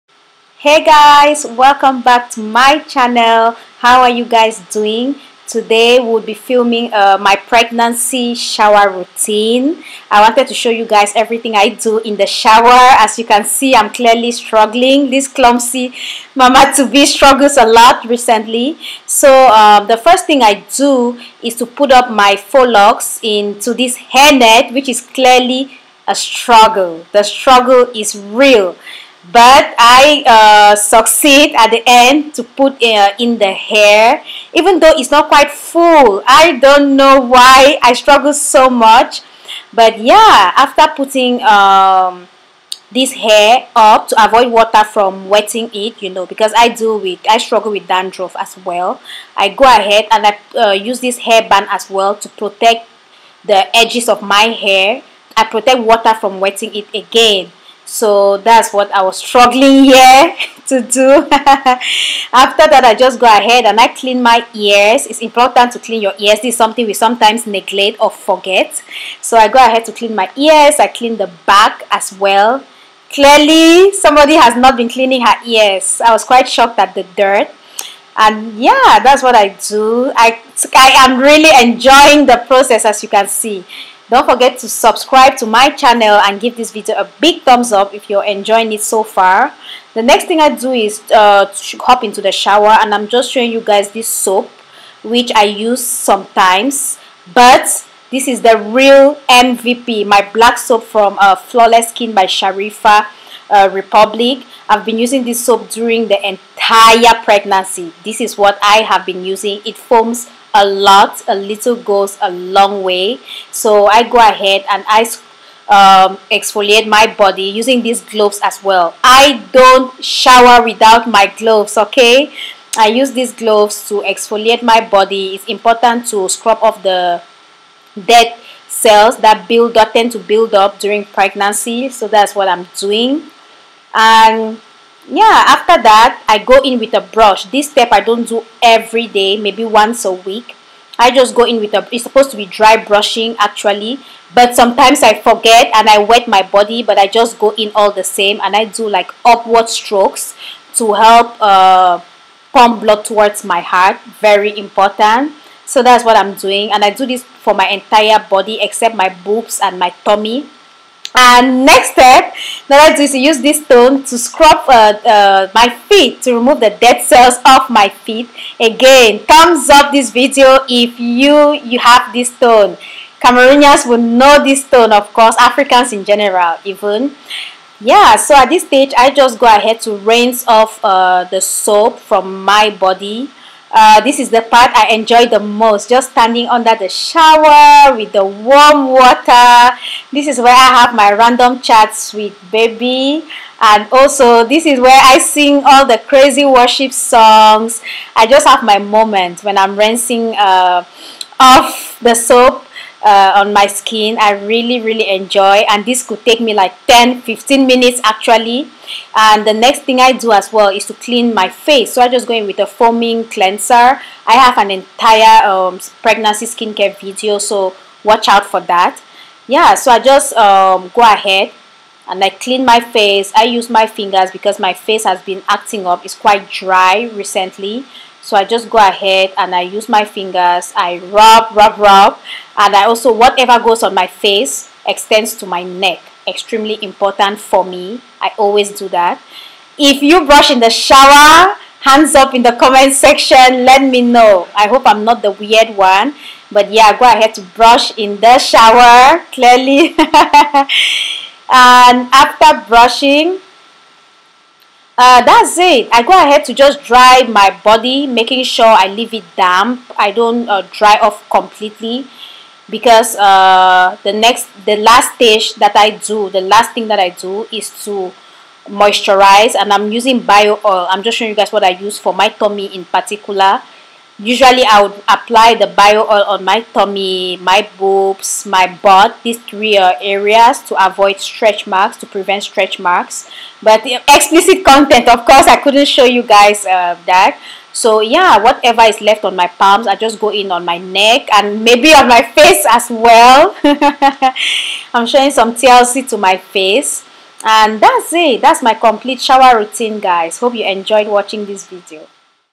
MY WINTER SHOWER ROUTINE WHILE PREGNANT + FEMININE HYGIENE TIPS
MY WINTER SHOWER ROUTINE WHILE PREGNANT + FEMININE HYGIENE TIPS! Hey guys, I'm sharing with you all my winter shower routine while pregnant today. Its easy to follow and i show all the steps i follow in the bathroom. Let me know any feminine hygiene tips you practice that you think will suit a pregnant woman. Love always.